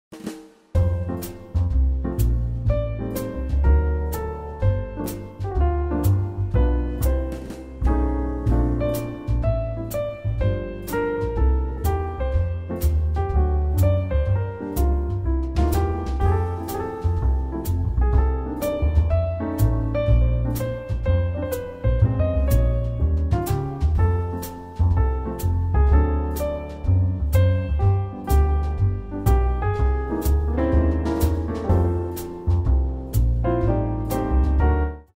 Homemade Lightened Up Eggnog
An easy, delicious DIY eggnog recipe that's refined sugar free. Recipe and video created for Egg Farmers of Canada and 80twenty.